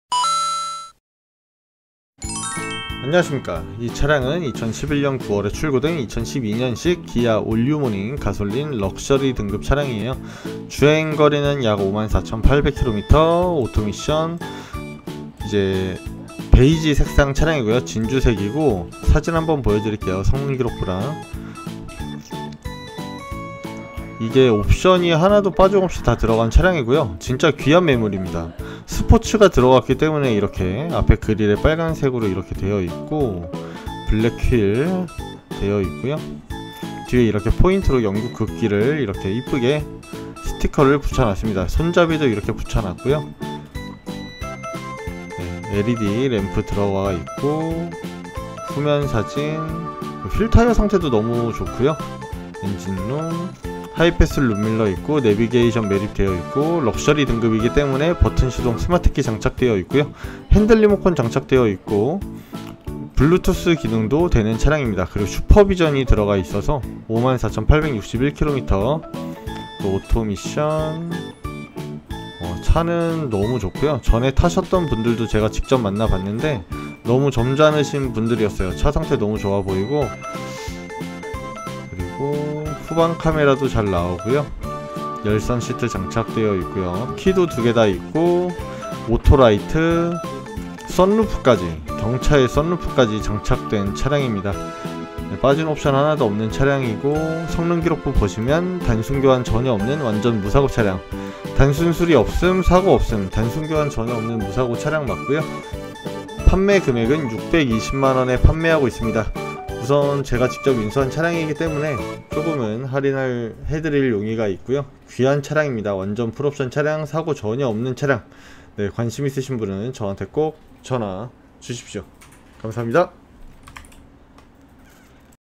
(딜러전산실매물) 열선핸들 썬루프에 스포츠팩까지 들어간 올뉴모닝?? 7117
감사합니다

620만원 올뉴모닝 7117